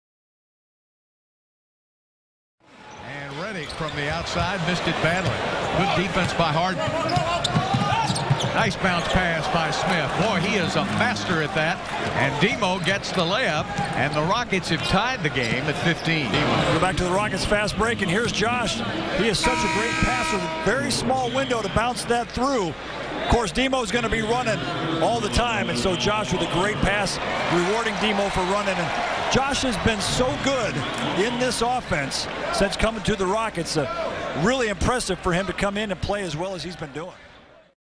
Josh Smith perfect fast break bounce pass to Motiejunas
Josh Smith runs the break and threads the needle with a perfect bounce pass between two defenders to Donatas Motiejunas in stride in the Houston Rockets game against the Los Angeles Clippers on 2/11/2015.